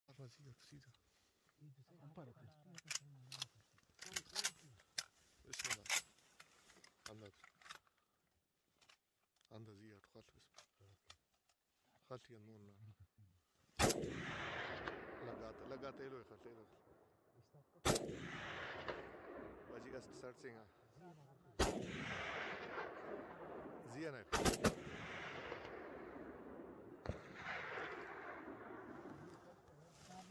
Ibex hunting || hunter missed the shots.
This time Ibex was so close and very Lucky.